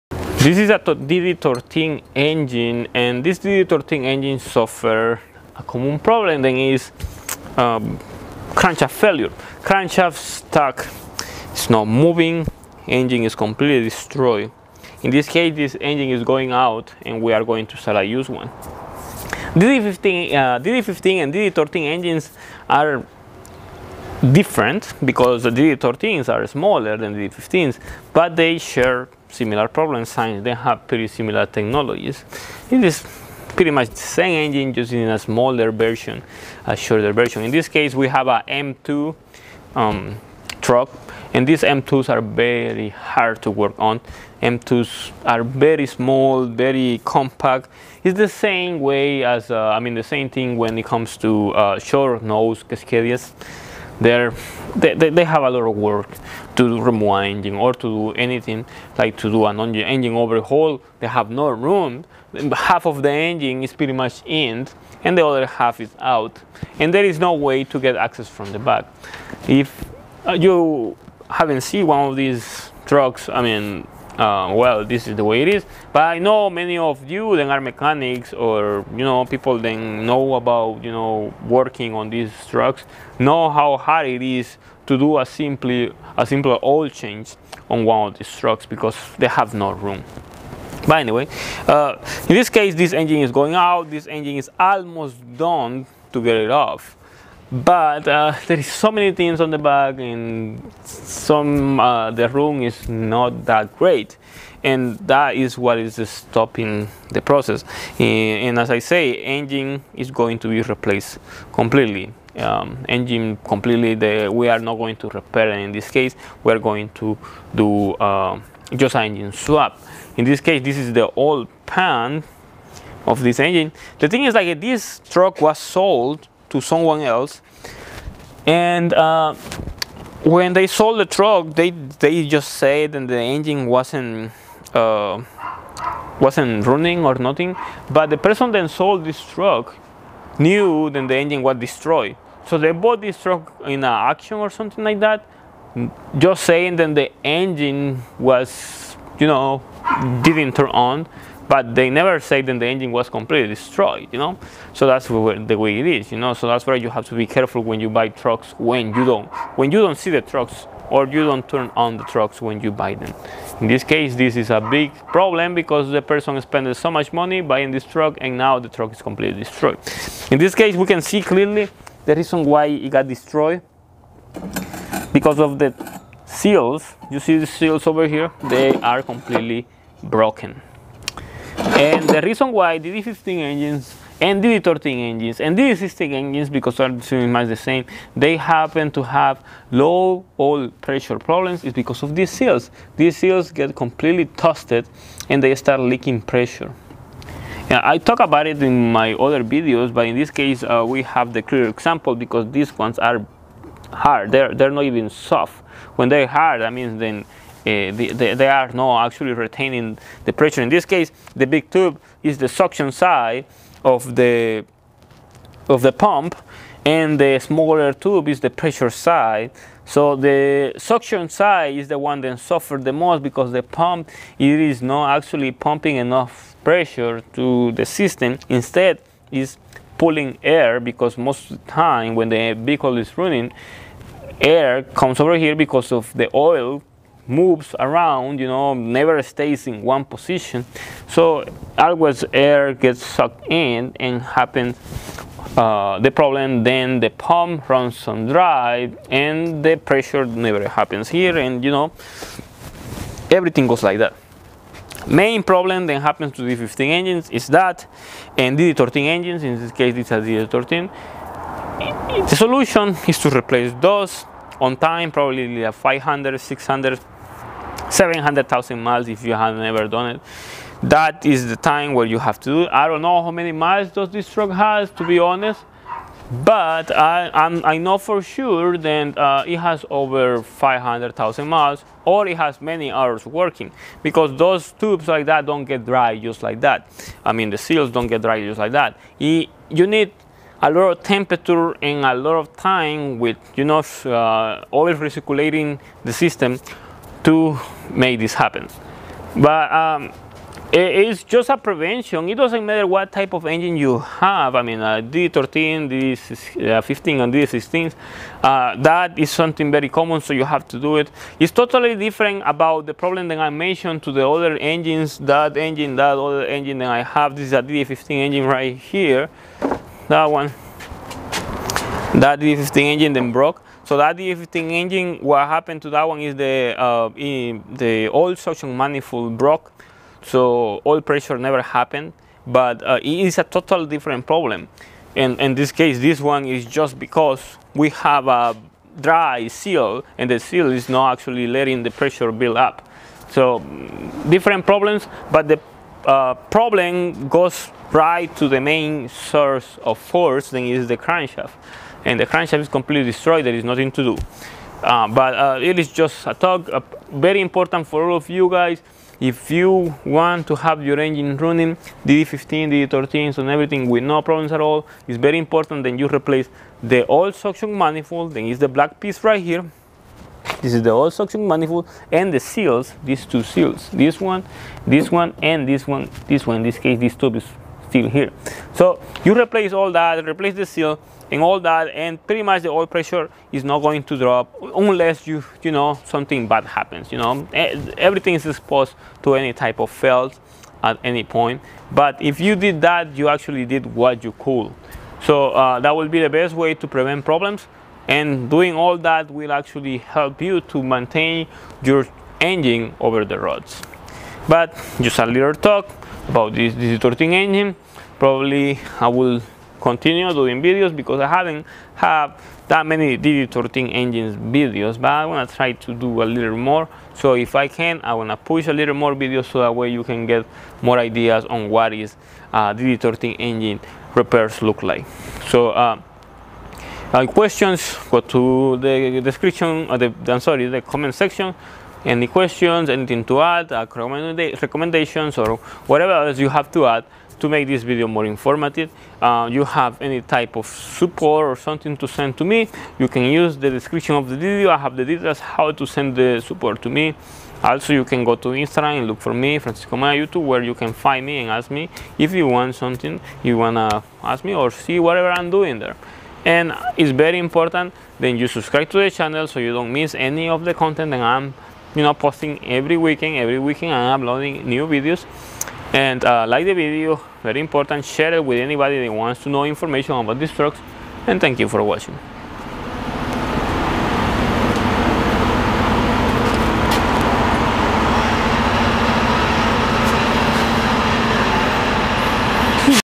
Freightliner M2 DD13 low oil pressure bad oil pump orings broken oil manifold crankshaft damage
WARNING!!: ⚠️⚠️⚠️

Warning!!! ⚠️⚠️⚠️

Topics discussed:
low oil pressure problem
what causes this issue
way to repair this problem
how to when the bearings are damage
what is the cost of this repair
DD13 oil pressure problems
steps to repair this problem

symptoms of the fault are:
low oil pressure warning
engine does not start
red stop light on
damage rod or main bearing
metal debris in oil filter
engine does not rotate
engine makes loud noise

Shop talk:
 most of the time DD13 engines are having problems with low oil pressure caused by broken oil suction manifold or damage oil pump orings. it is important to replace this every 600k miles or less to prevent problems with your engine.

Francisco Amaya

Via

OR

I.E. euros

Question!?